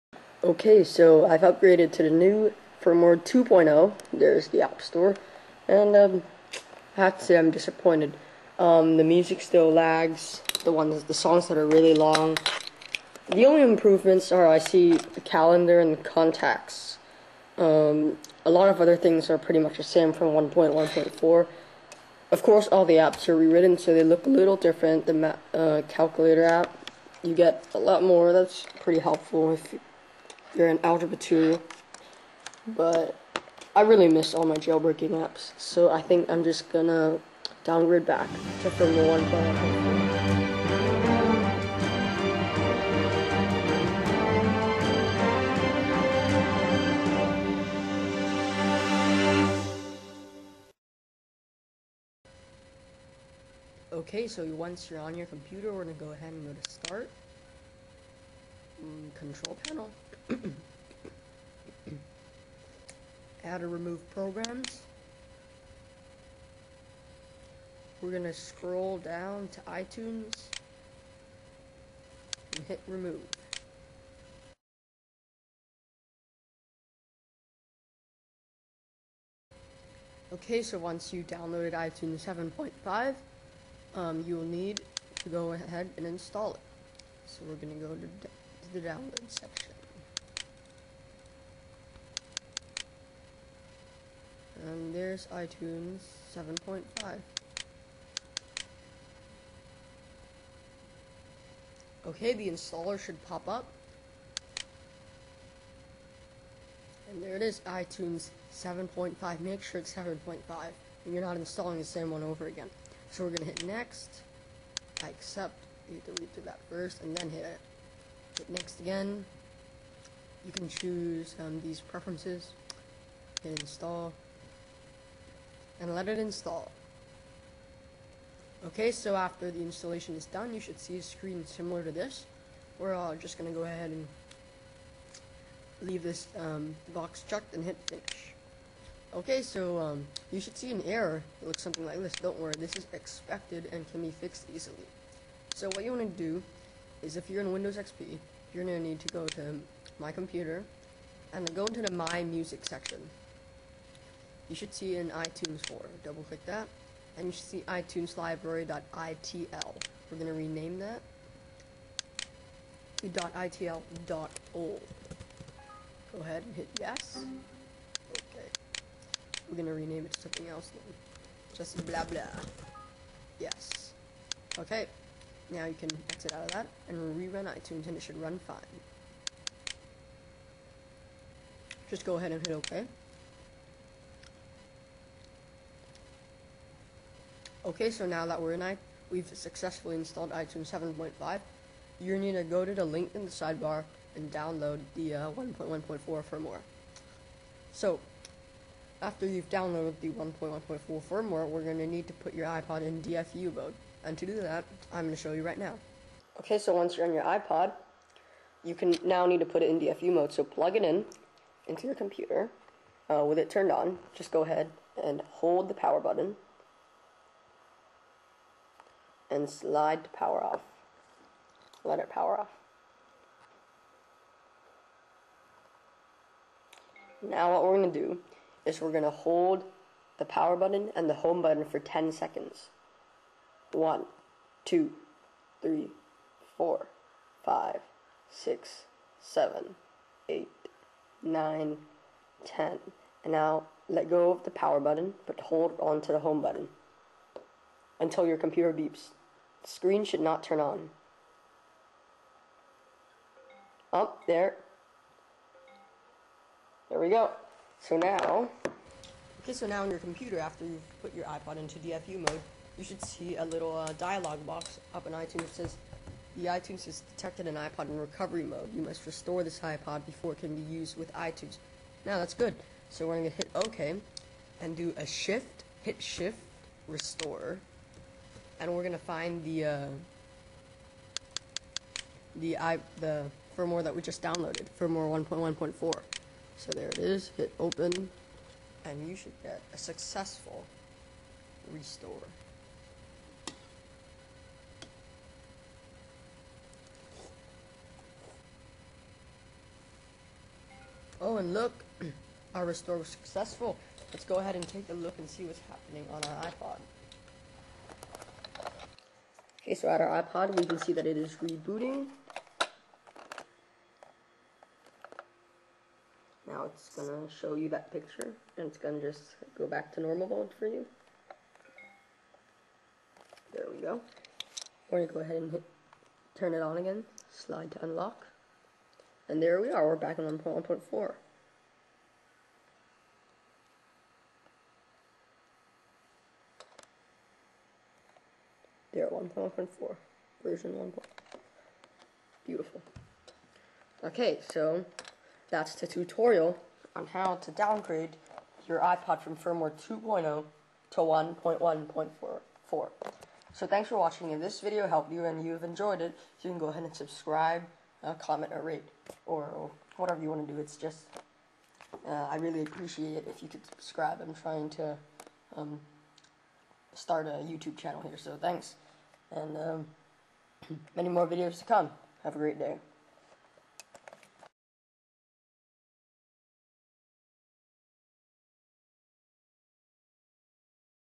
Downgrade 2.0 to 1.1.4 for iPod Touch/iPhone Easy Tutorial!
THANKS!

A cool tutorial on how to downgrade your ipod touch or iphone to 1.1.4 if you don't like 2.0.

UPDATED LINKS SORRY!!!
^^Those should include iPhone too!